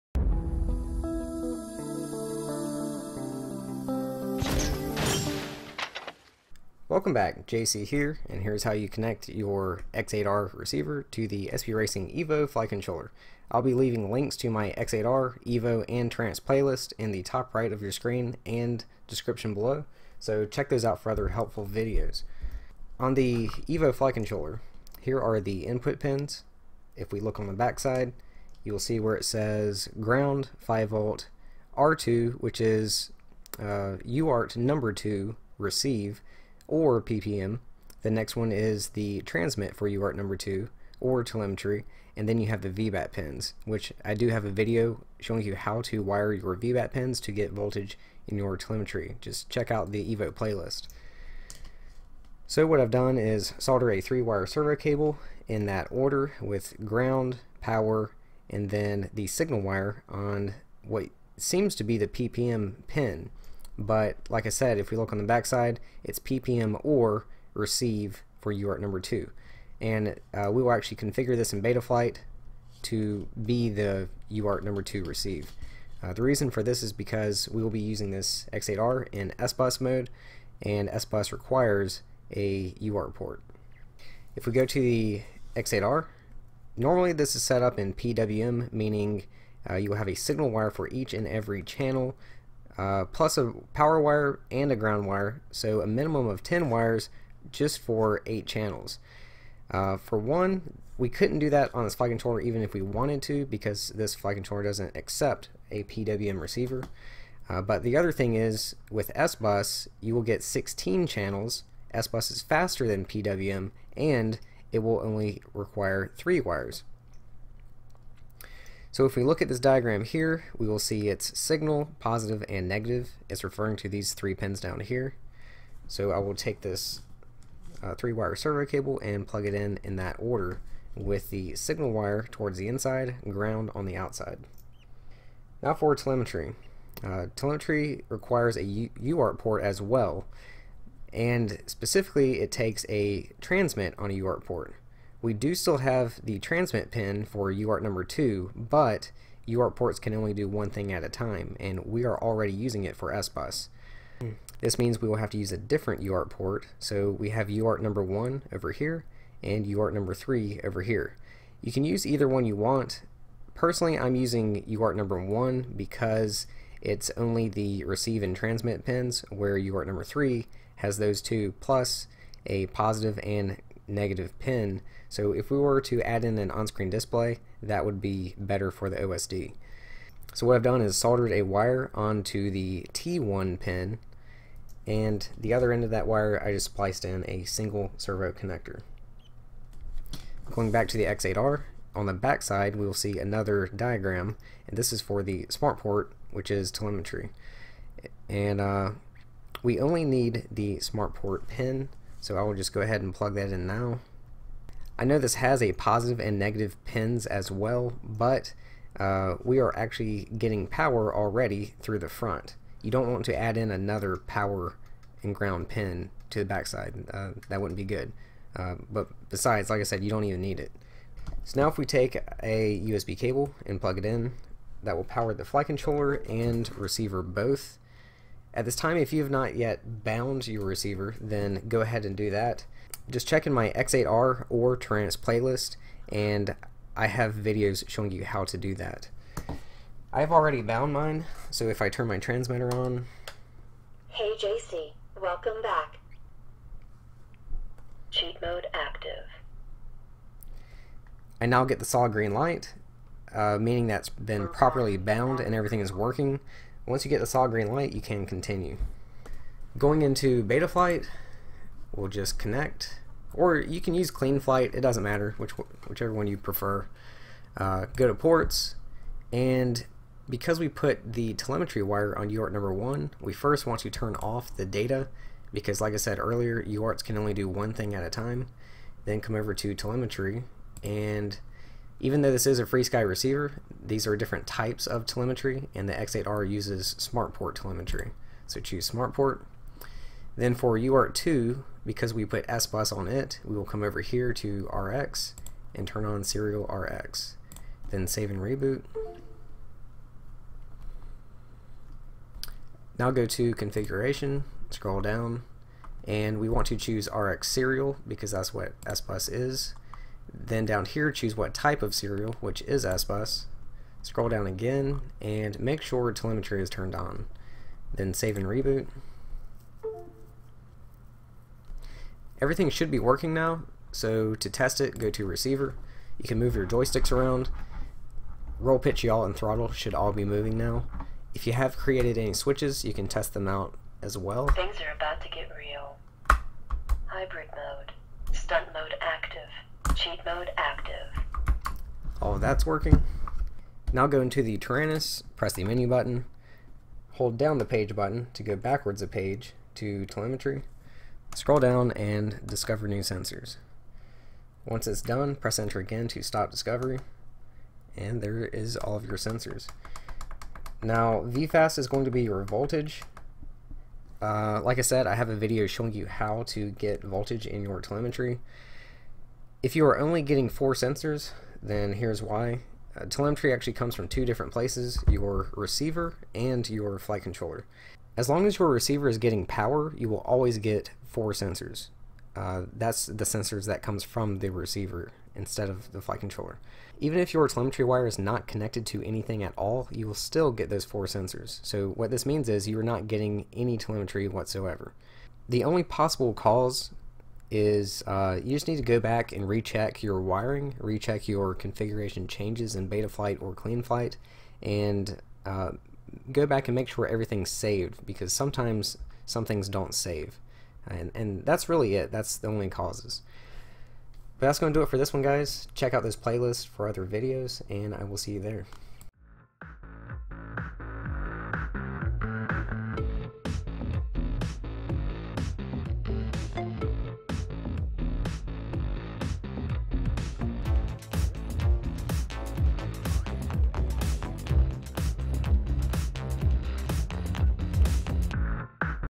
SPRacing Evo & X8R: Connect & Telemetry
This is how to connect the FrSky X8R receiver in Sbus mode to the Seriously Pro Racing Evo flight controller. I cover how to wire everything, set up Betaflight, and get telemetry in the Taranis.

Taranis Q X7 (Amazon)
Taranis Q X7 (Banggood)
Taranis Q X7 (Ebay)